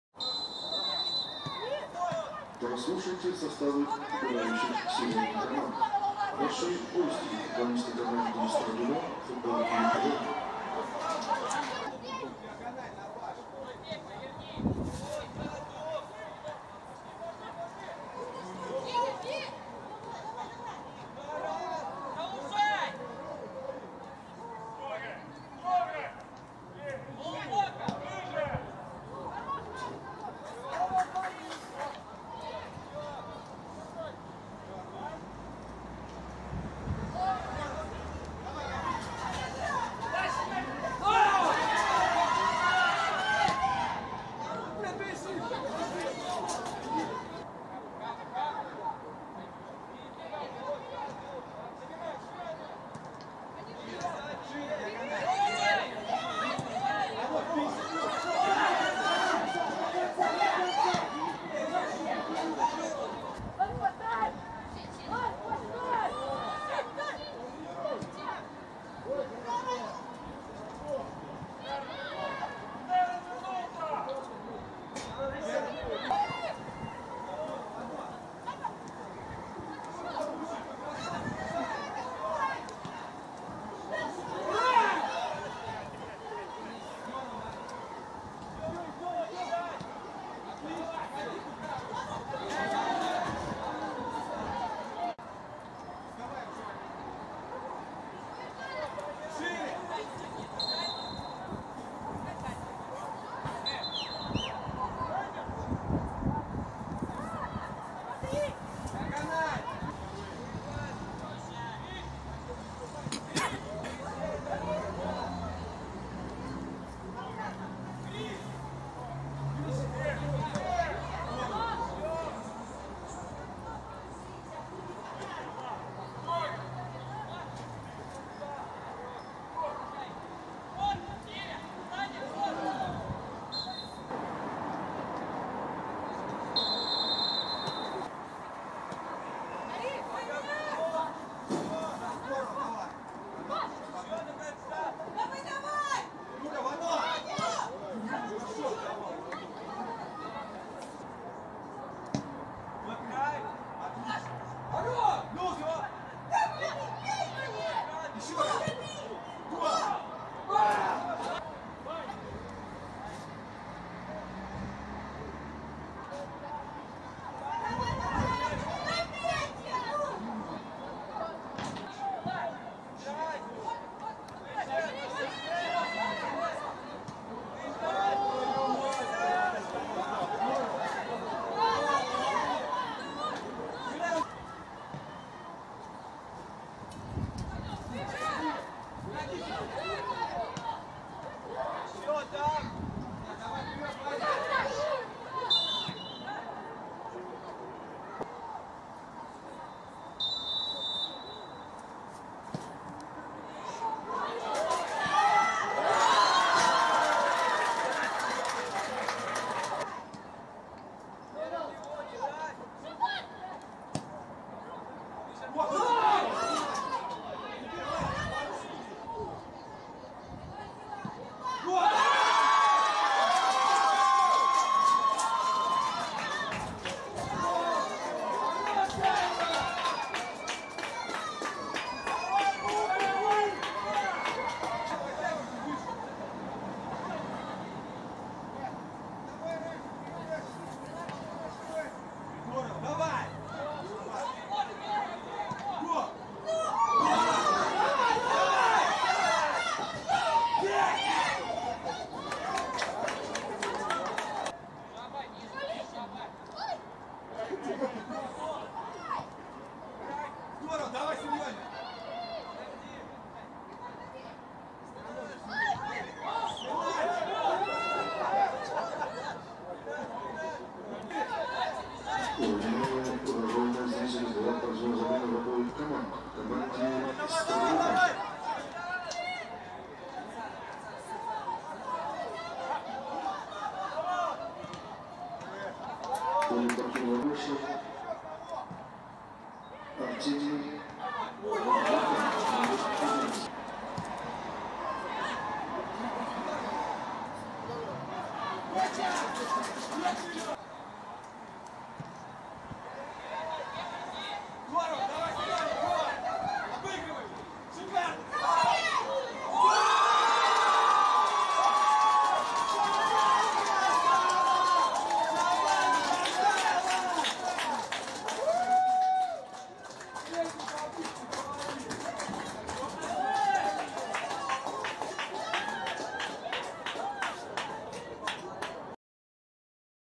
"Химки" - "Строгино" 2008 | Летнее Первенство 2021. Клубная лига | Обзор | 1:2
15 тур.